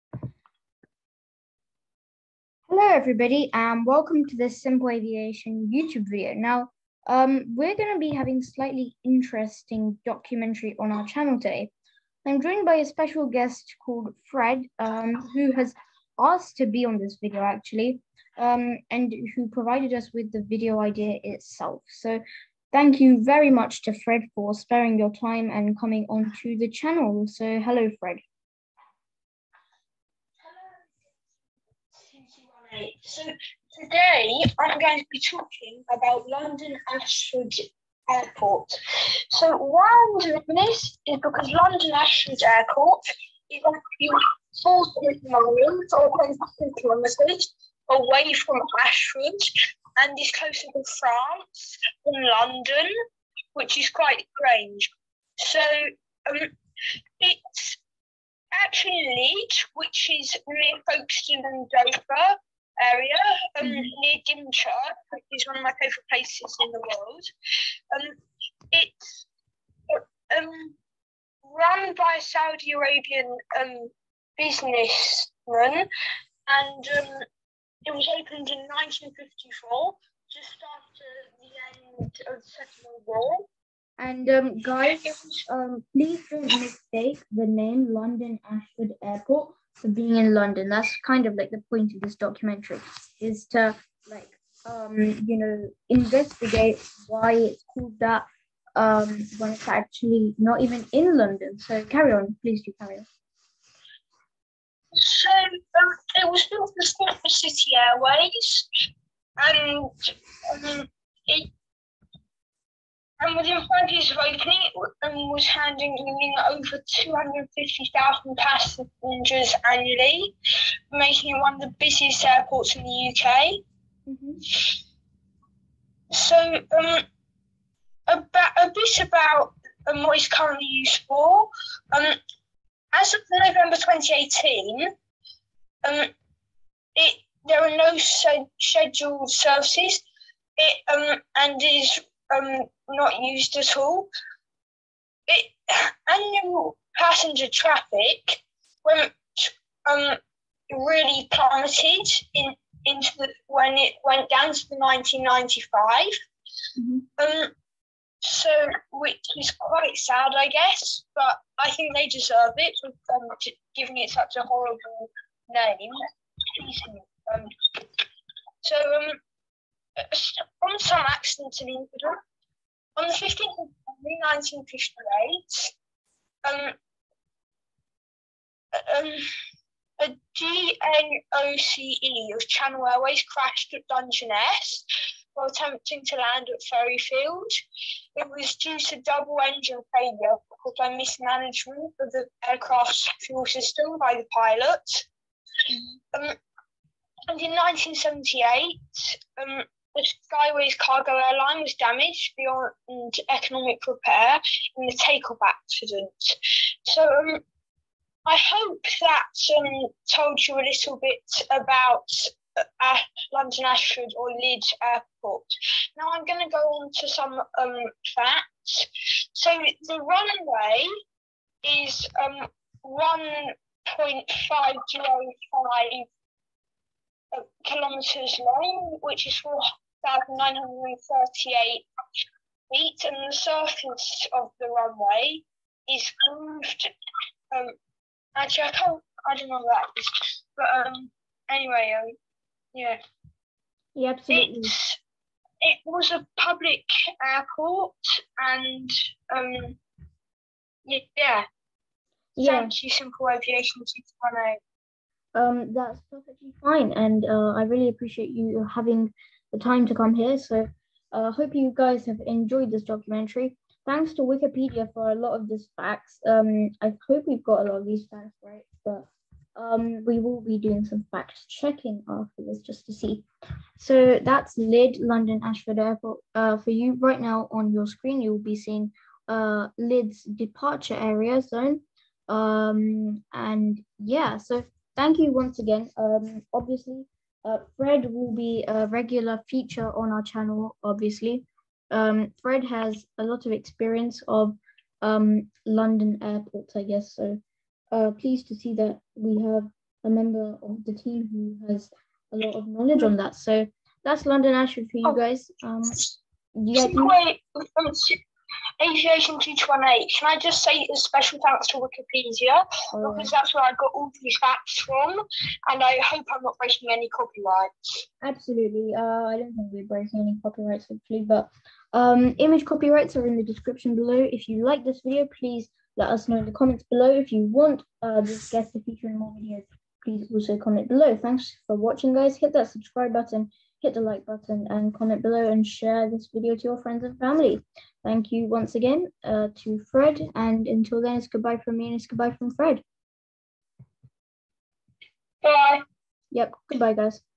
LYDD Airport- A London Ashford Airport Documentary
I'm joined today by "Fred" who talks about the peculiarly named London Ashford Airport! Why is it peculiarly named? Watch the video to find out!

Image copyrights-

Description
English: LAA runway
Author LyddGC

Description
English: Lydd Airport ( London-Ashford )
Date 12 August 2011
Author Brian Chadwick
Permission

Copyright credit for 2nd image- Alamy

Facts copyright- WikiPedia